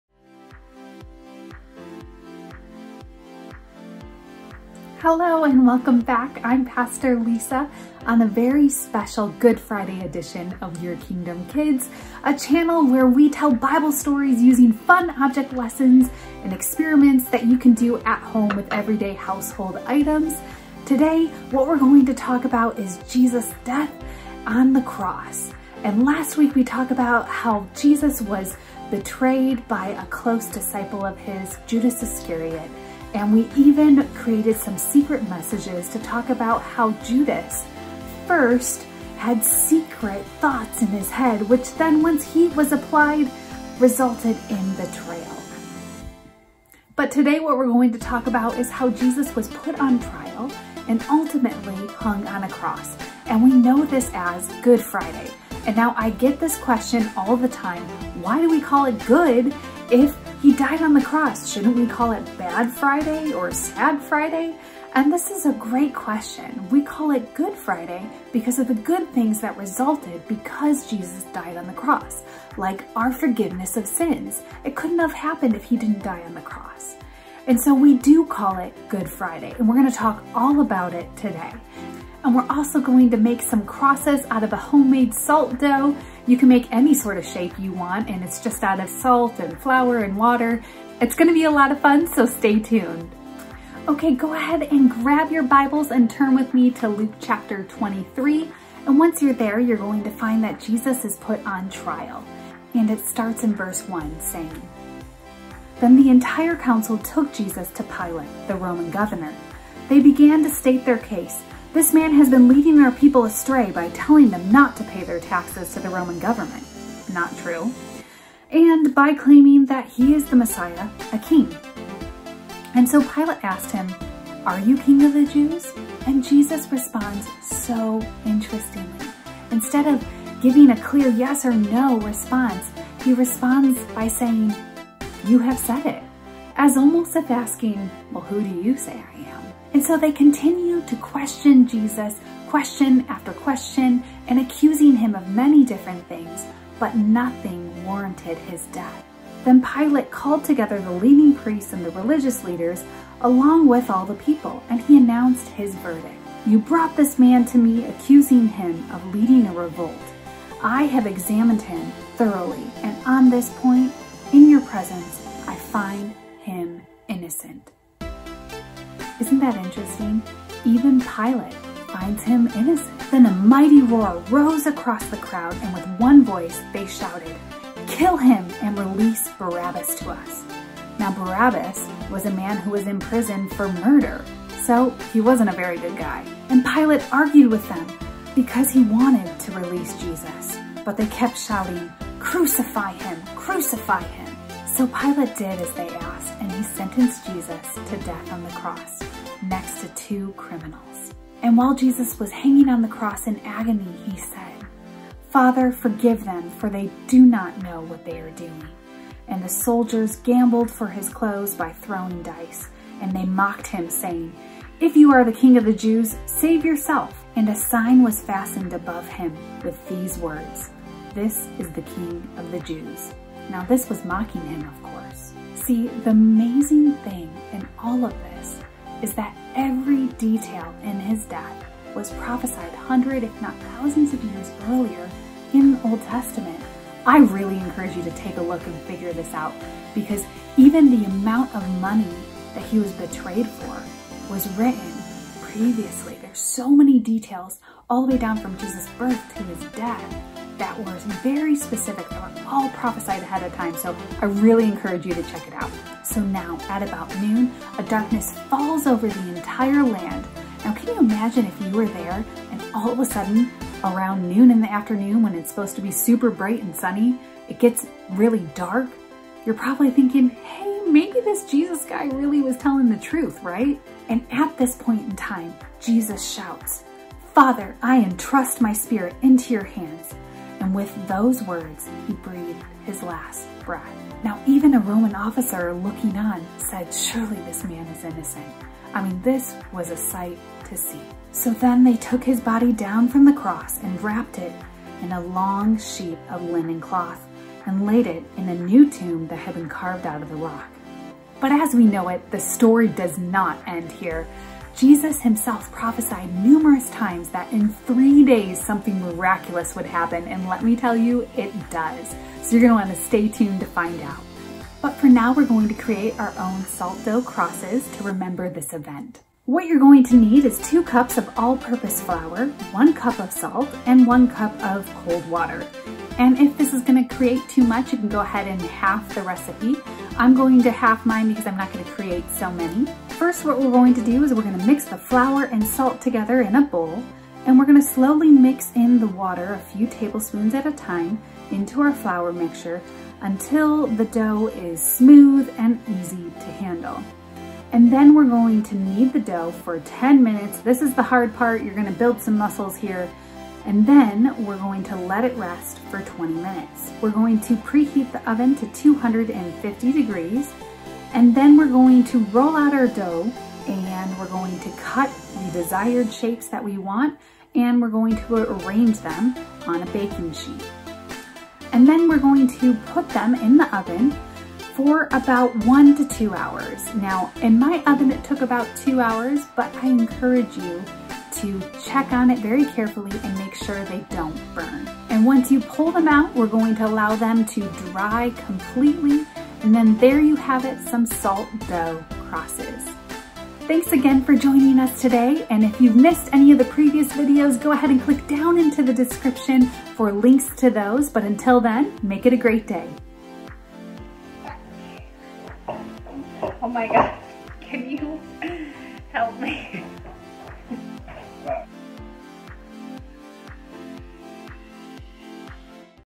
Good Friday Story for Kids + How to make Salt Dough Ornaments + Luke Ch. 23
Good Friday Story for Kids - How to make Salt Dough Crosses, Luke chapter 23 the death of Jesus